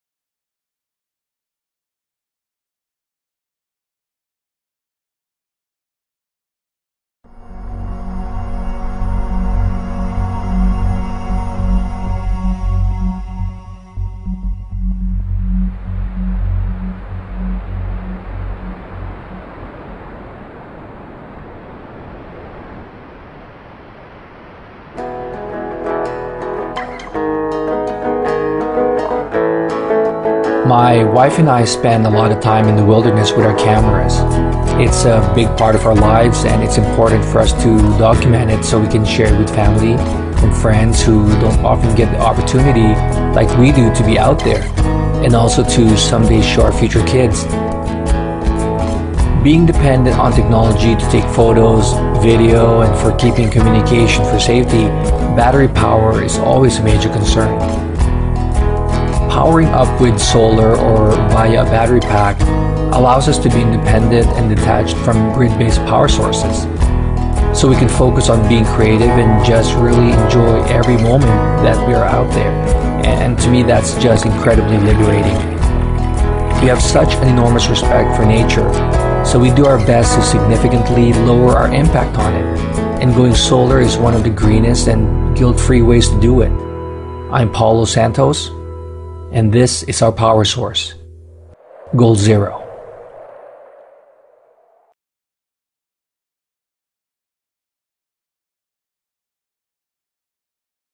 Guide 10 Plus Mobile Kit
The Guide 10 Plus Mobile Kit is a pocket-sized wonder that's perfect for keeping all your small gadgets charged and ready to go, wherever you go! Place this amazing solar kit inside your car or backpack and you'll never have to waste your money with Alkaline batteries again.